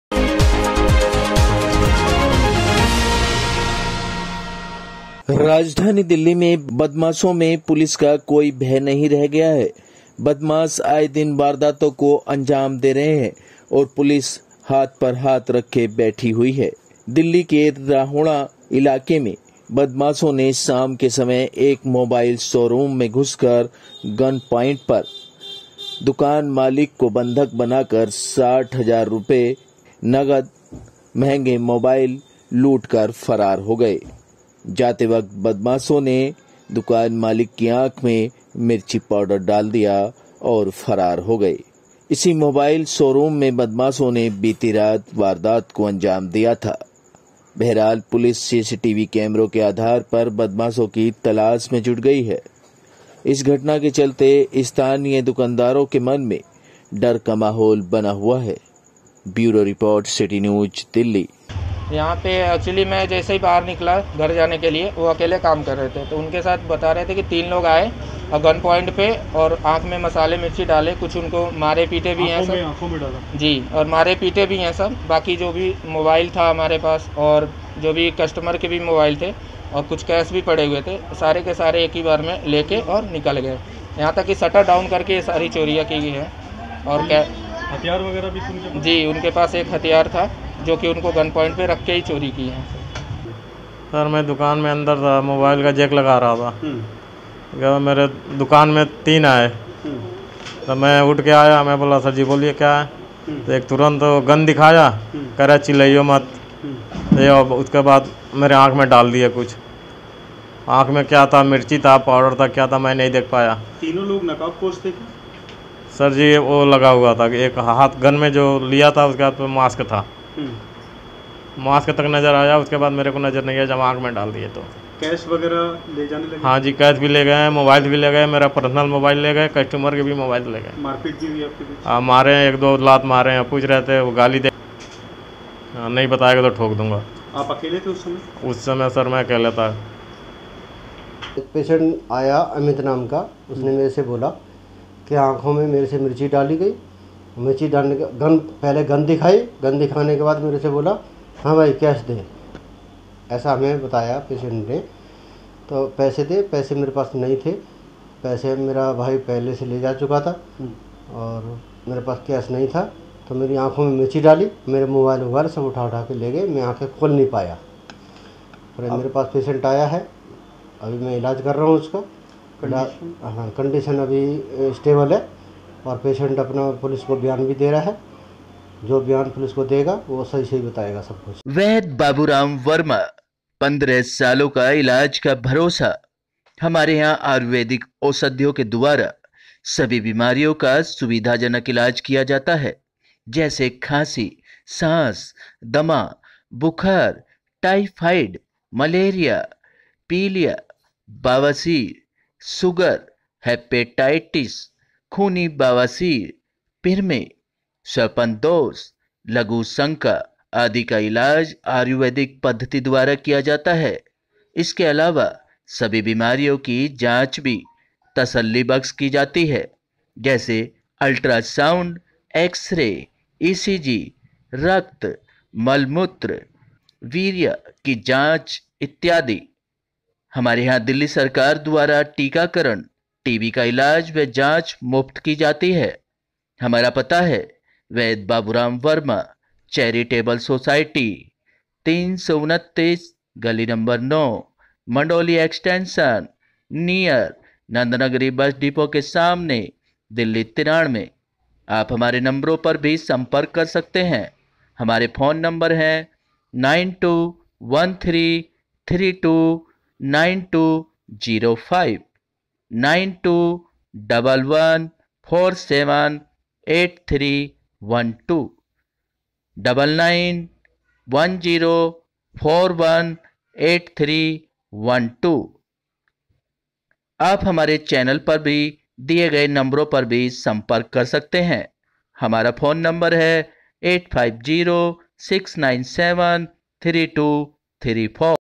CITY NEWS DELHI -बदमाशों ने मोबाइल शोरूम के अंदर घुस कर हत्यारों के बल पर लूट की घटना को दिया अंजाम
बदमाशों ने मोबाइल शोरूम के अंदर घुस कर हत्यारों के बल पर लूट की घटना को दिया अंजाम । बदमाशों ने दुकान मालिक को बंधक बनाकर 60 हजार नगद और महंगे मोबाइल की घटना को दिया अंजाम । राजधानी दिल्ली के रणहौला इलाके का मामला । पुलिस मामले की जांच में जुटी । स्थानीय दुकानदारों में रोष । बदमाश आए दिन वारदातों को दे रहे हैं अंजाम